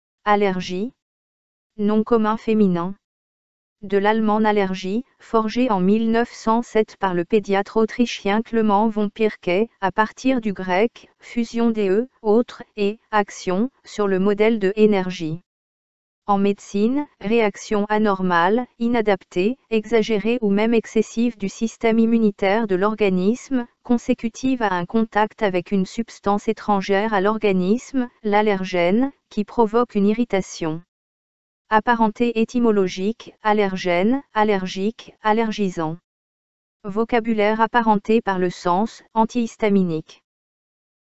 Allergie | Dictionnaire français audio
Définition du terme Allergie.
De l’allemand Allergie, forgé en 1907 par le pédiatre autrichien Clemens von Pirquet, à partir du grec, fusion de (« autre ») et (« action »), sur le modèle de énergie.
En Médecine, Réaction anormale, inadaptée, exagérée ou même excessive du système immunitaire de l’organisme, consécutive à un contact avec une substance étrangère à l’organisme (l'allergène) qui provoque une irritation.
Apparentés étymologiques : allergène, allergique, allergisant.
Vocabulaire apparenté par le sens : antihistaminique.